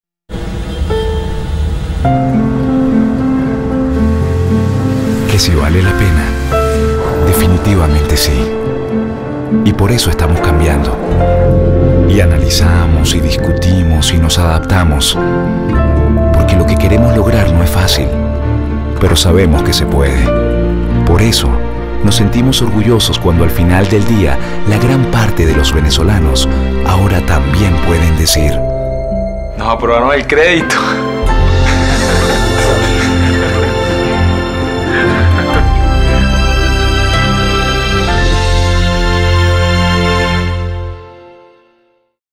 Banco de Venezuela - Nos aprobaron el crédito
Comercial de Imagen para el Banco de Venezuela hecho por La Cocina Publicidad